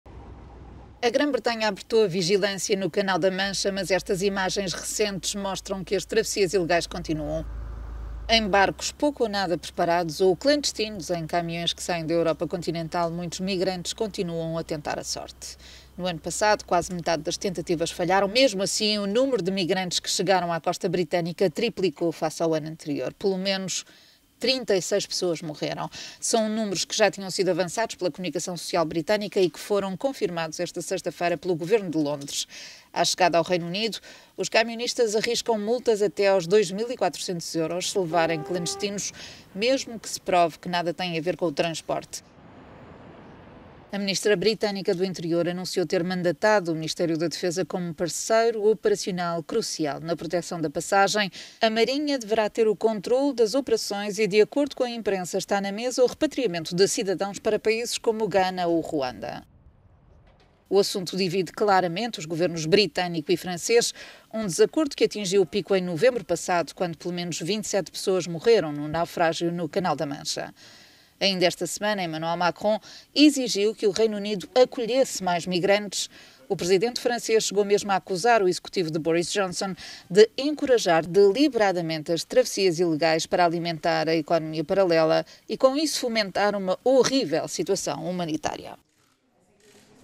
Rota ilegal de imigração continua a passar pelo Canal da Mancha
Britânicos reforçaram vigilância sem dar ouvido aos apelos franceses para uma política enquadrada de migração